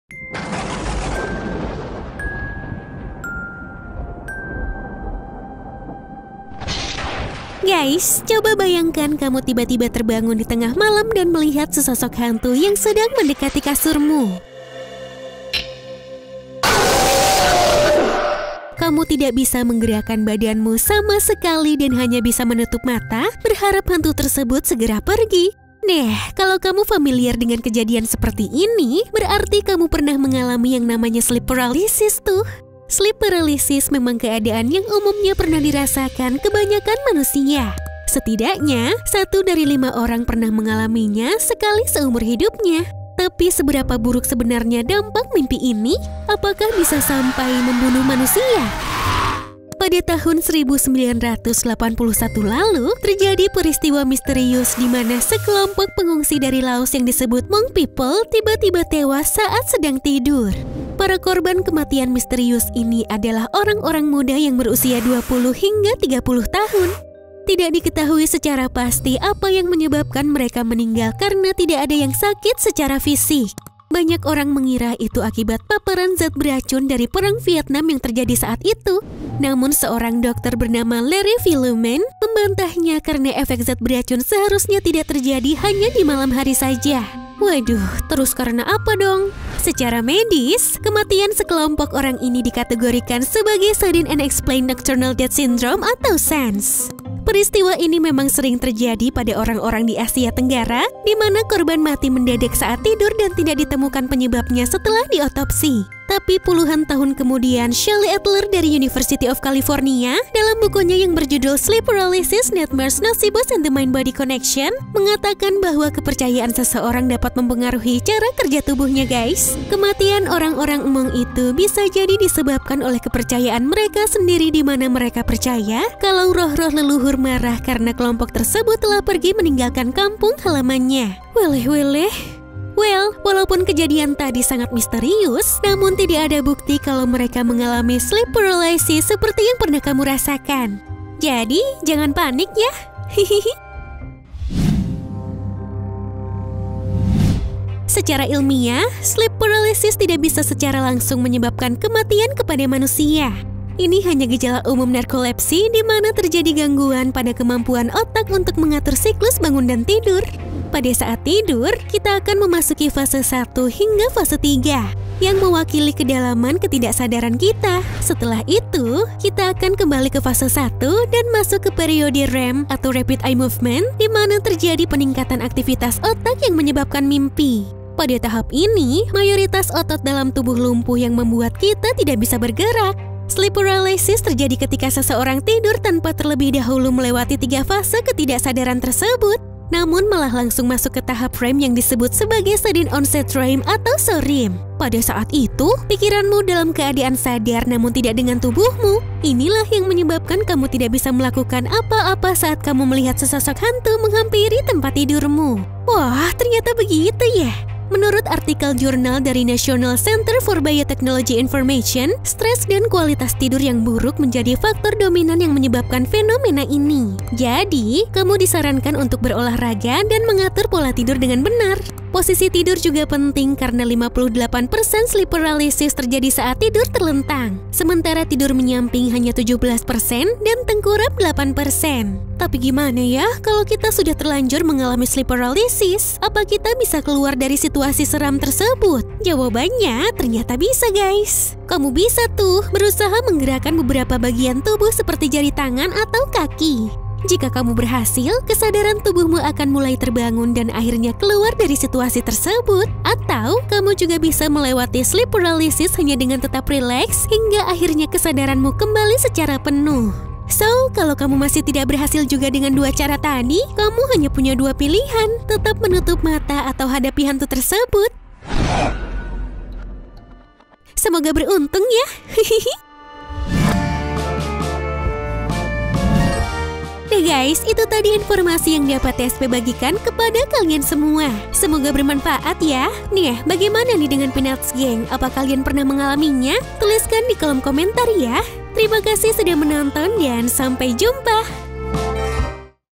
[ENG SUB] Can We Die from Sleep Paralysis?
Guys, imagine if you suddenly wake up in the middle of the night and see the figure of a ghost approaching your bed. You can’t move your body at all and you can only close your eyes hoping the ghost will just go away. If this sounds familiar it means you may be experiencing something called sleep paralysis. Sleep paralysis is actually something many people have experienced. At least one in five people experience it at some time in their lives. But how bad can the effects of it be? Can it be fatal?

Indonesia Translation :

Guys, coba bayangkan kamu tiba-tiba terbangun di tengah malam dan melihat sesosok hantu yang sedang mendekati kasurmu. Kamu tidak bisa menggerakan badanmu sama sekali dan hanya bisa menutup mata berharap hantu tersebut segera pergi. Nah kalo kamu familiar dengan kejadian seperti ini berarti kamu pernah mengalami yang namanya sleep paralysis. Sleep paralysis memang keadaan yang umumnya pernah dirasakan kebanyakan manusia. Setidaknya satu dari lima orang pernah mengalaminya sekali seumur hidupny. Tapi seberapa buruk sebenarnya dampak mimpi ini? Apakah bisa sampai membunuh manusia?